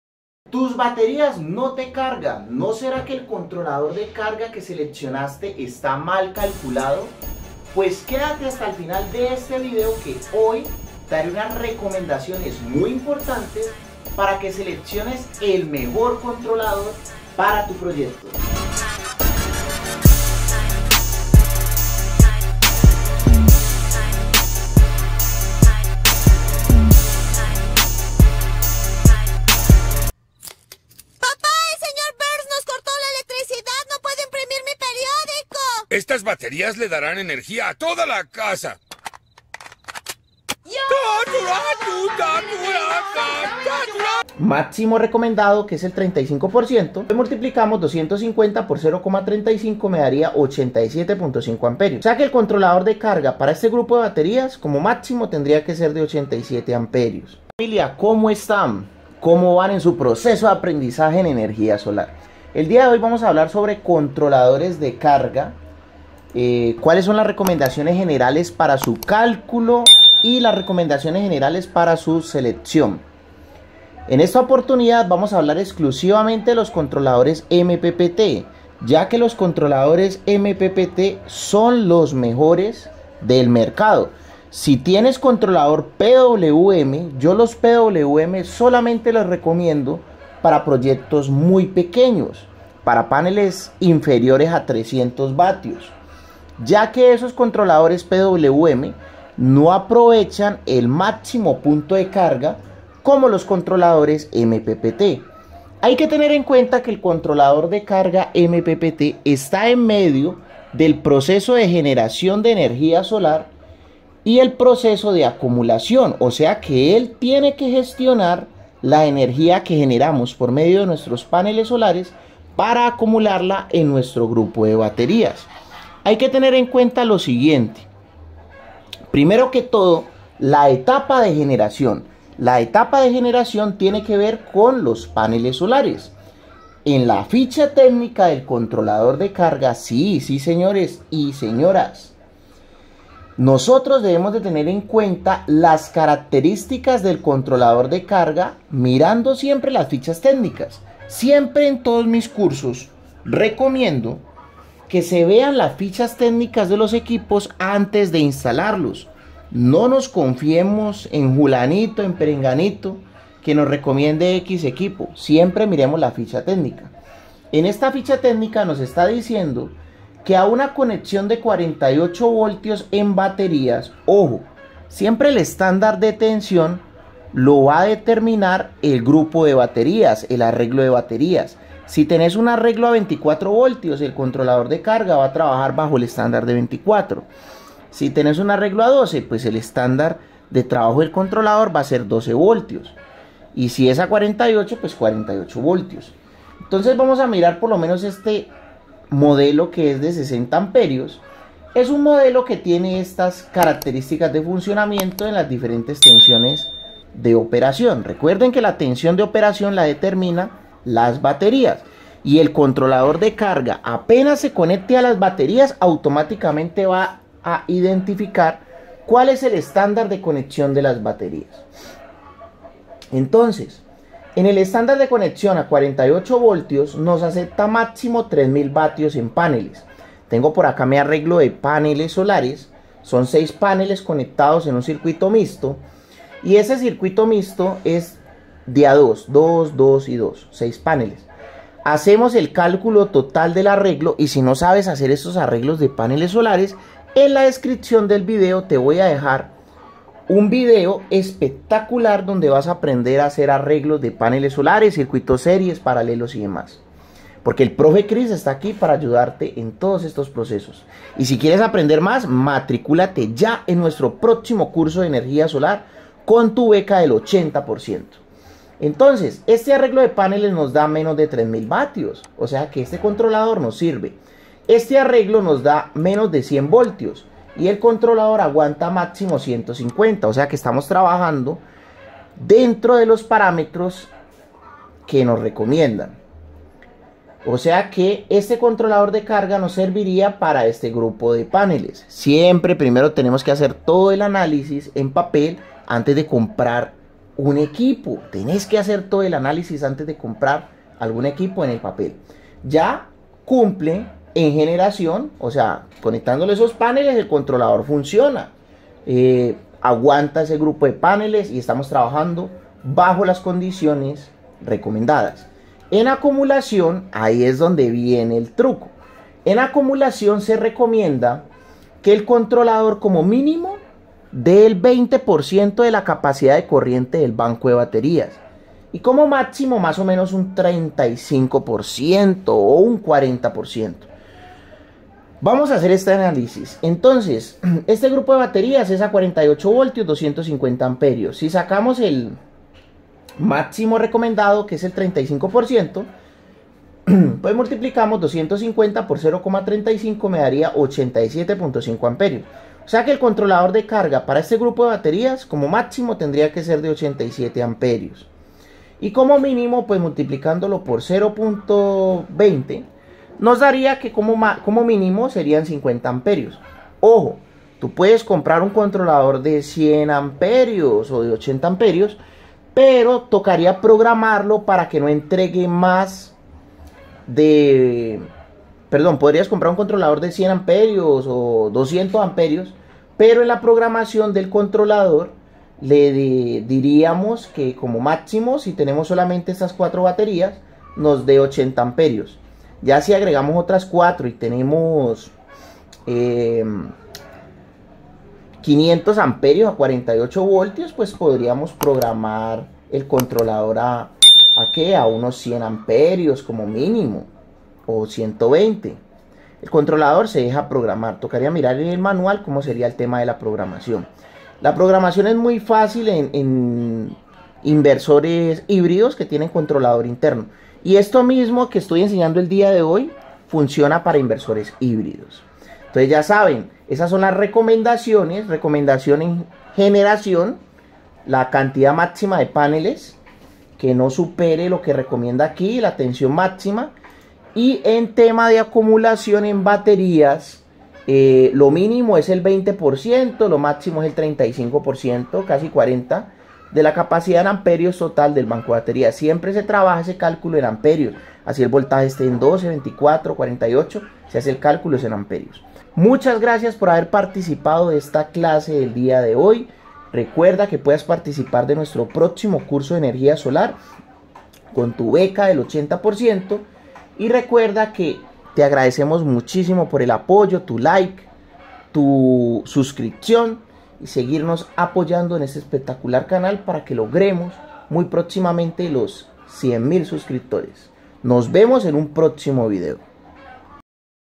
✅Batería no me carga controlador malo???
Tu batería no se carga? mal calculo de controlador???

participa de nuestro próximo curso con tu beca del 80%🧑‍🏫🧑‍🏫🧑‍🏫

venta de productos solo en Colombia
asesorías a nivel mundial cotiza la tuya